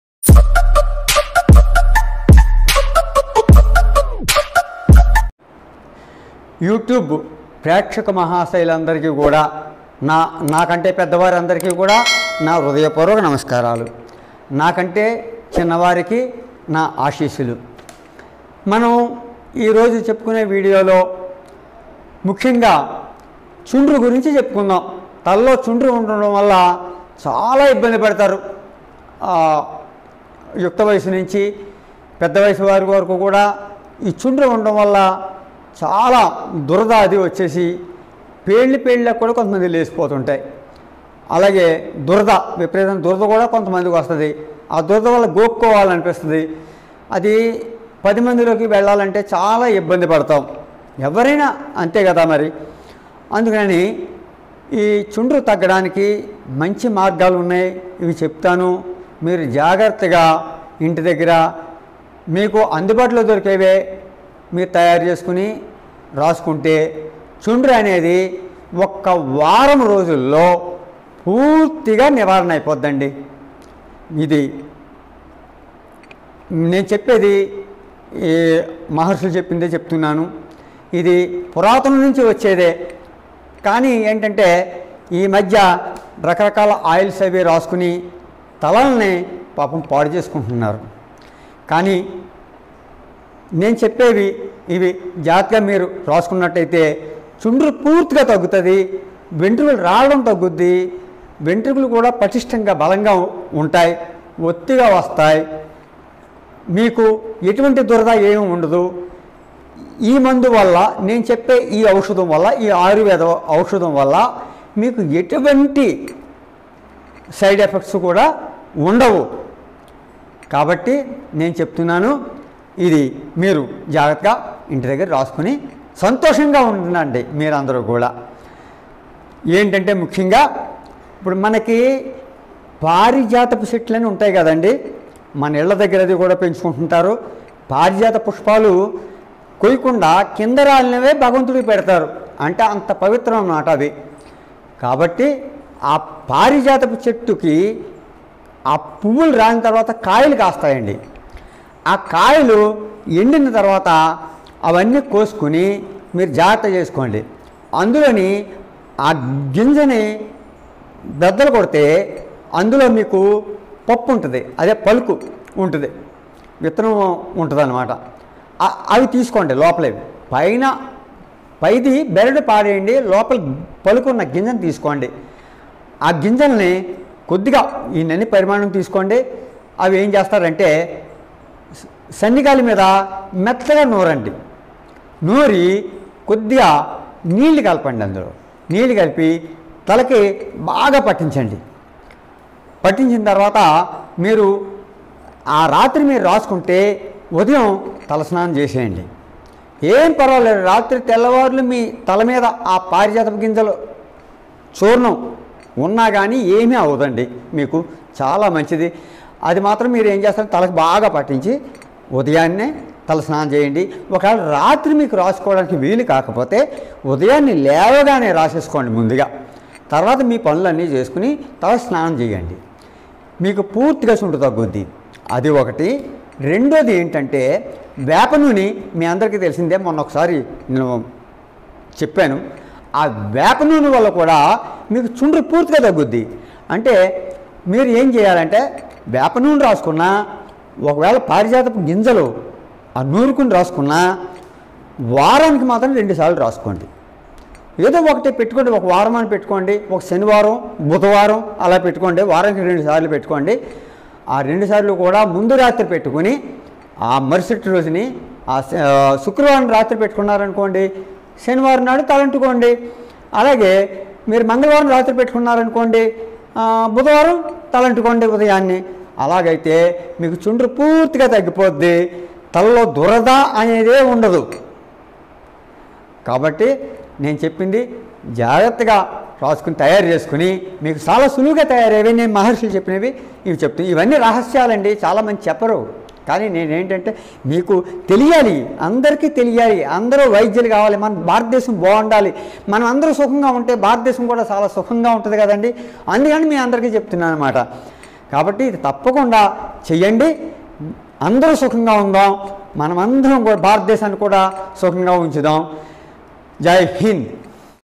How to get rid of dandruff from your hair???
items required:
1.parijatha seeds
2.vepa nune.